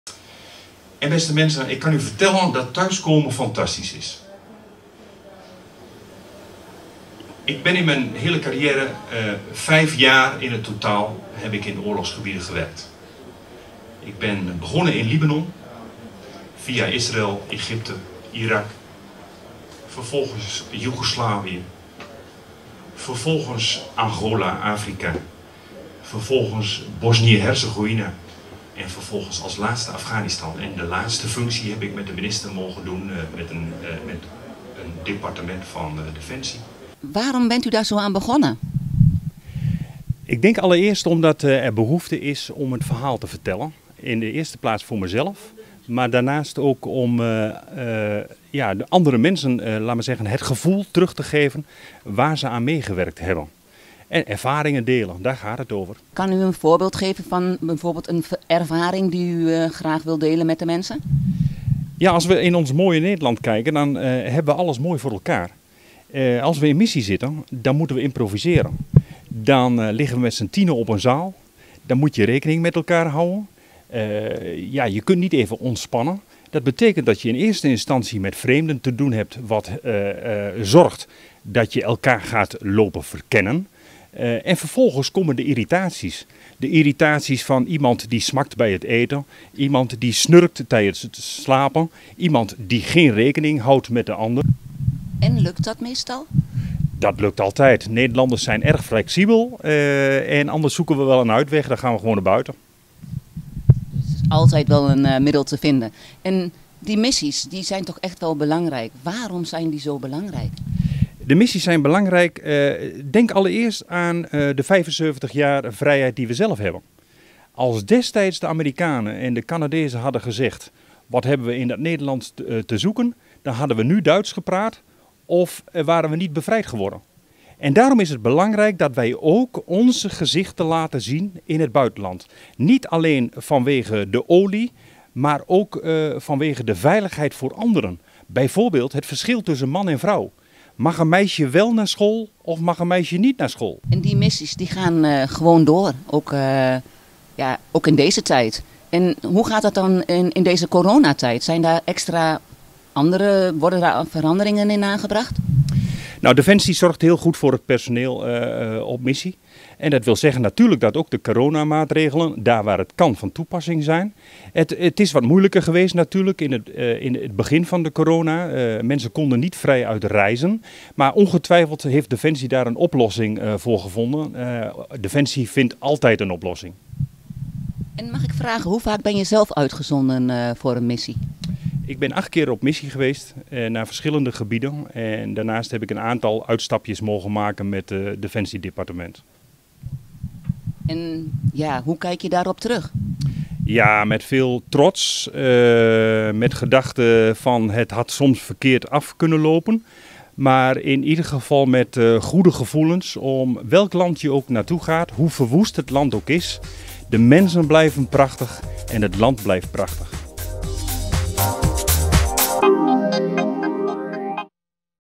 Lezing over uitzendingen spreekt aan
Tien uitzendingen naar oorlogsgebieden maakten dat Albert Zomer vele boeiende verhalen kan vertellen over wat het betekent om een dergelijke missie mee te maken, maar ook over het land en zijn gewoontes weet hij veel door te geven. Onlangs gaf hij een lezing voor de Ouderen Vereniging Almelo (OVA) in het Huis van Katoen en Nu.